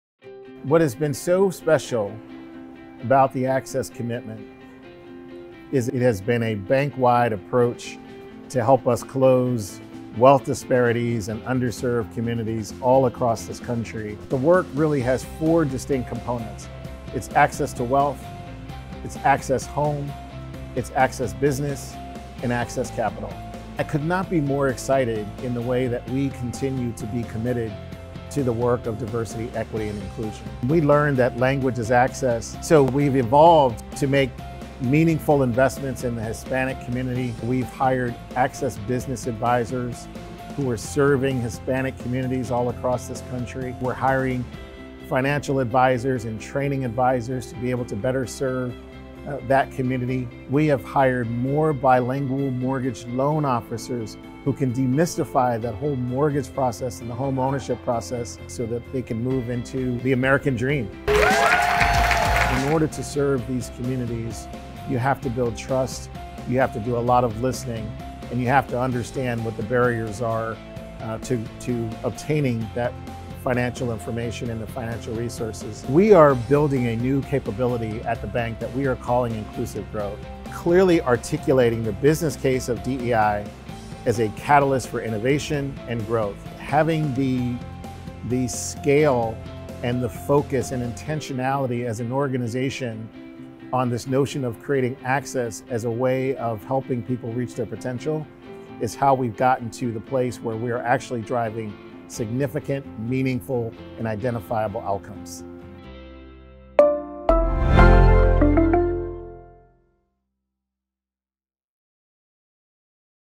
Continuing the work to help close the wealth gap for underserved communities
Three years ago, U.S. Bank launched U.S. Bank Access Commitment®, its long-term approach to help close the wealth gap for underserved communities, including communities of color, and redefine how we serve employees, clients and communities. It started with a focus on the Black community, where the persistent economic wealth gap in the United States is the largest*, and in 2023, expanded its focus to include the Hispanic community.